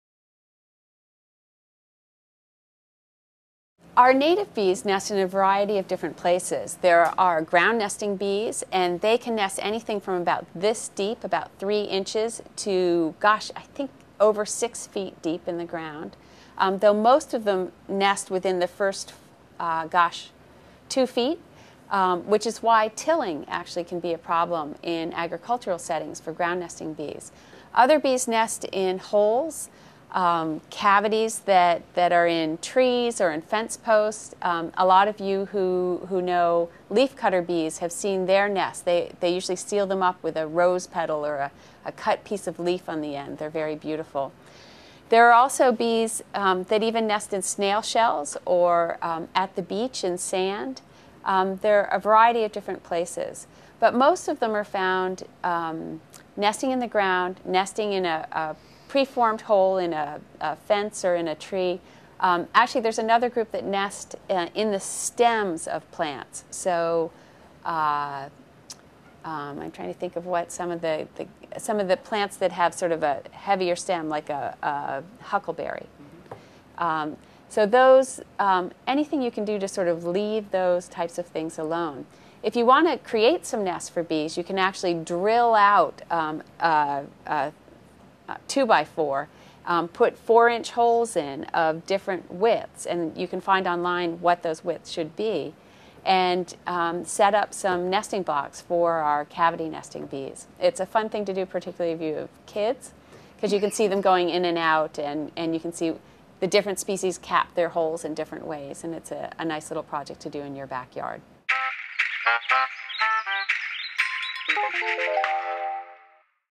Where bees nest - Citizen Science: Bee curious!
The Great Bee Count - a fun, easy and important Citizen Science effort! Help shape bee conservation efforts by planting bee-friendly plants and counting the bees in your garden this summer:

Check the Playlist tab for more videos about the Great Bee Count